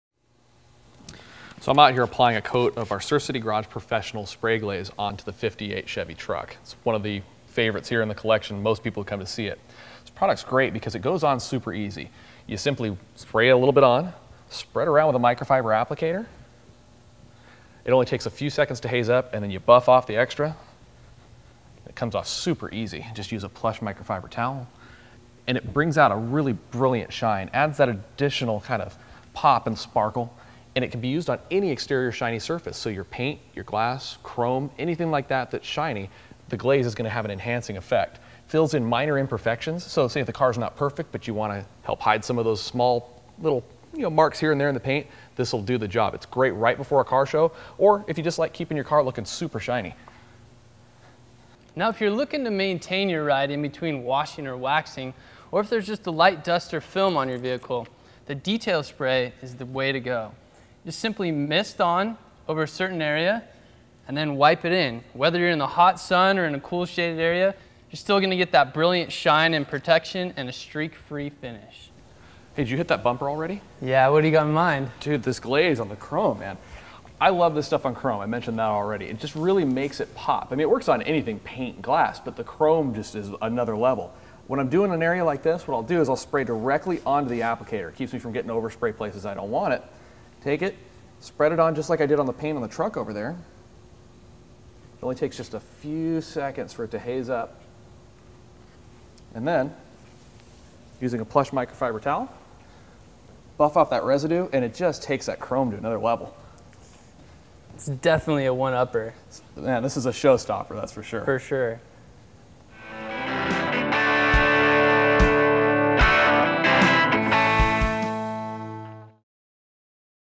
Enhancing Your Cars Finish
Surf City Garage Professional Surface Solutions presents two of our biggest secrets to a show winning shine - Polymer Detail Spray and Spray Glaze.